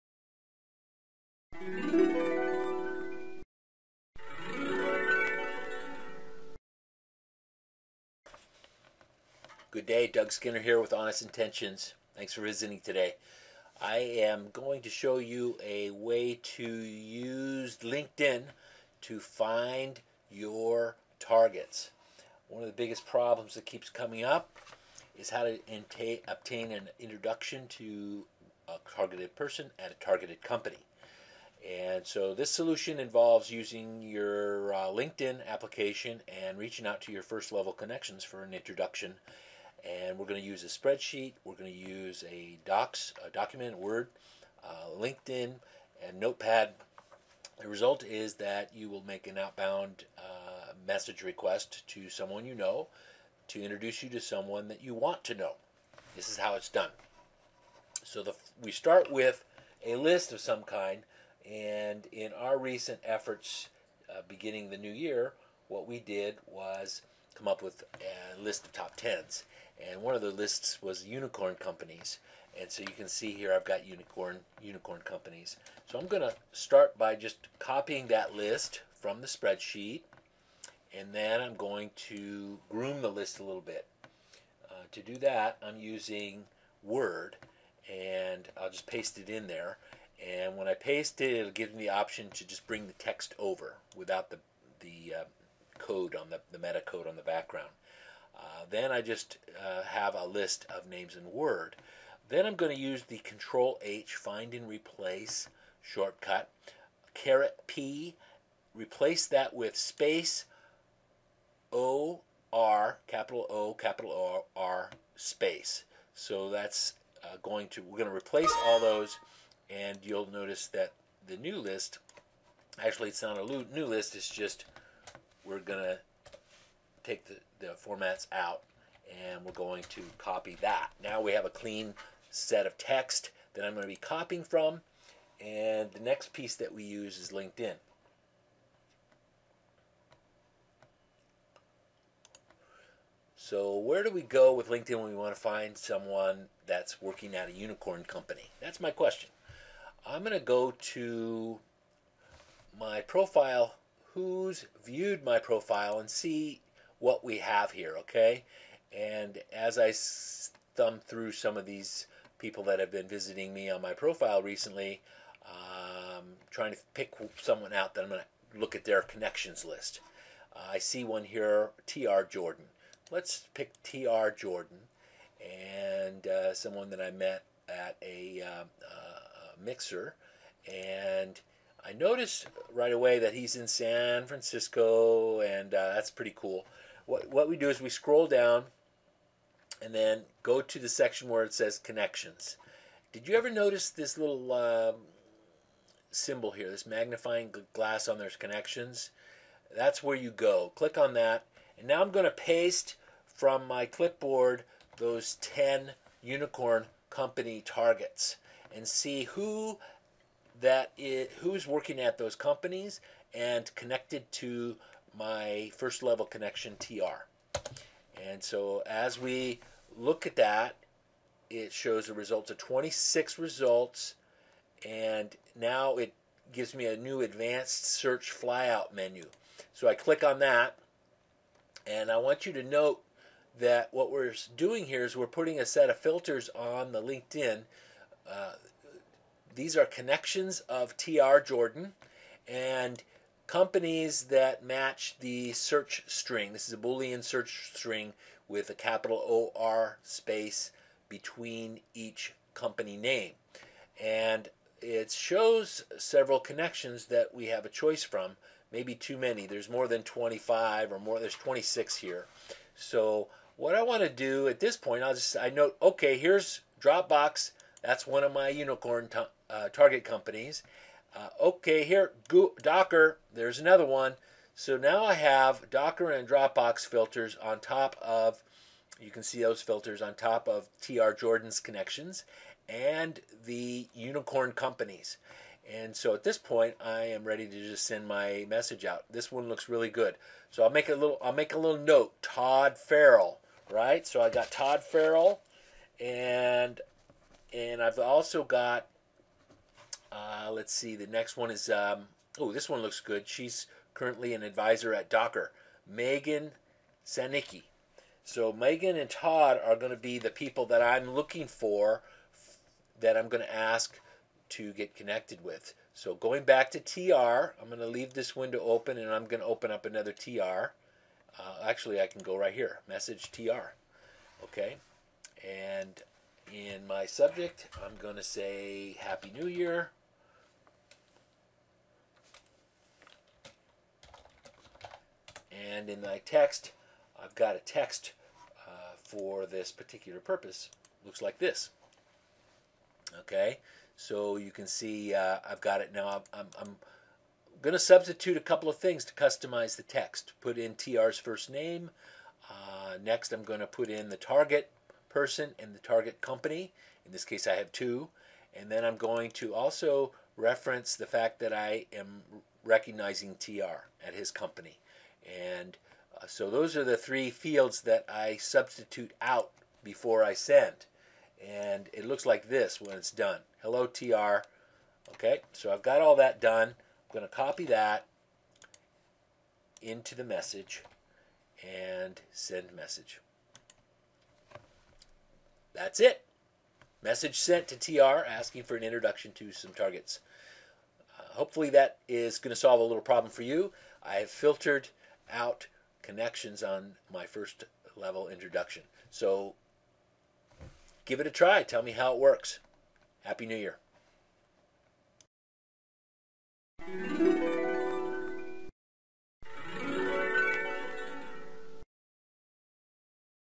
Finding Introductions from 1st Connections on LinkedIn
Use LinkedIn to find and get introduced to your targets. Searching for Unicorns?  A targeted person at a targeted company.  Result is you make an outbound message request to someone you know for an introduction to someone you want to know at a targeted company.